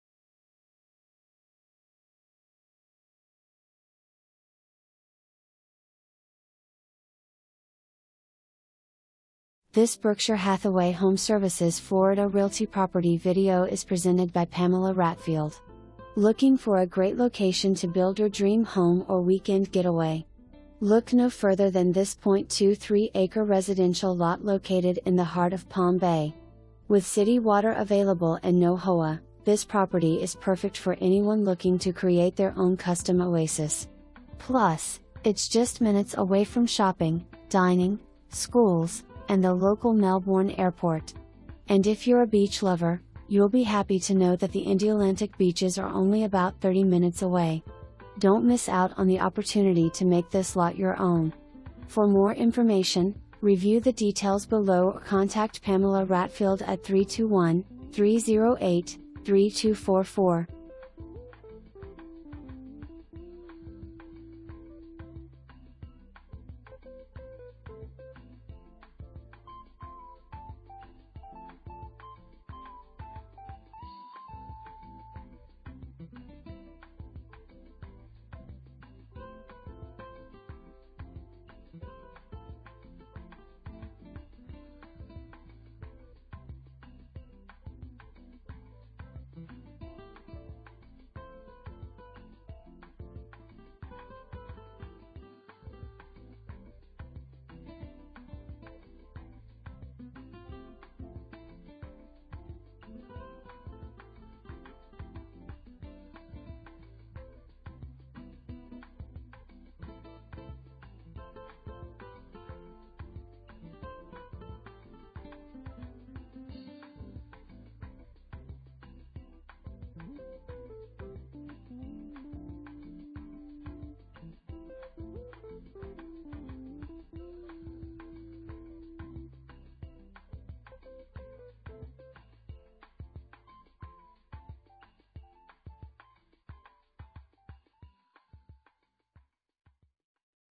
Berkshire Hathaway HomeServices Florida Realty - 971 Pace Drive NW
Looking for a great location to build your dream home or weekend getaway? Look no further than this .23 acre residential lot located in the heart of Palm Bay! With city water available and no HOA, this property is perfect for anyone looking to create their own custom oasis. Plus, it's just minutes away from shopping, dining, schools, and the local Melbourne airport. And if you're a beach lover, you'll be happy to know that the Indialantic beaches are only about 30 minutes away! Don't miss out on the opportunity to make this lot your own.

Pamela Ratfield
Berkshire Hathaway HomeServices Florida Realty